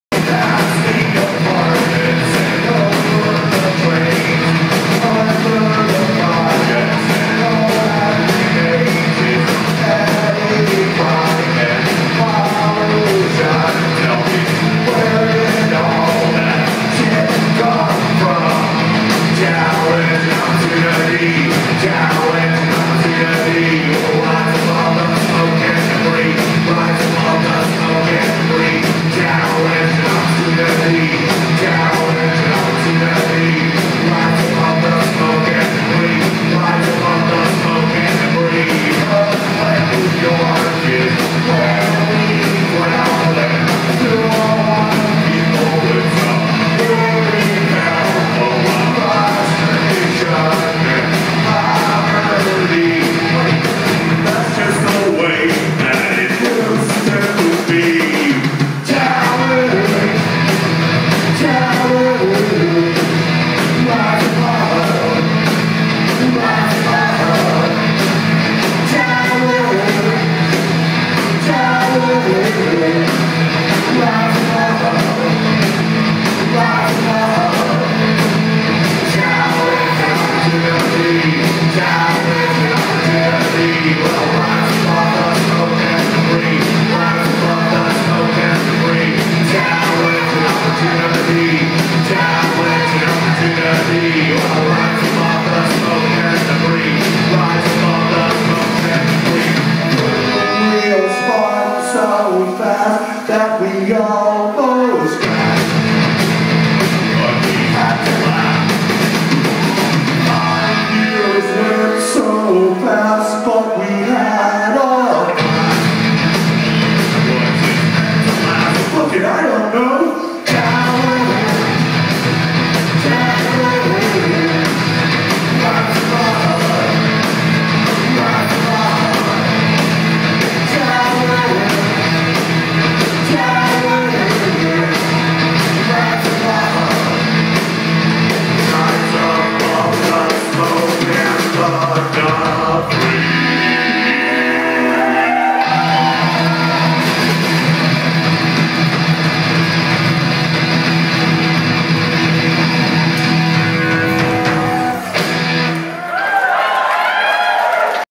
Shellshag, Music Hall of Williamsburg, 6/14/11
Shellshag opened for Japanther's 10th Anniversary celebration at Music Hall of Williamsburg, 6/14/11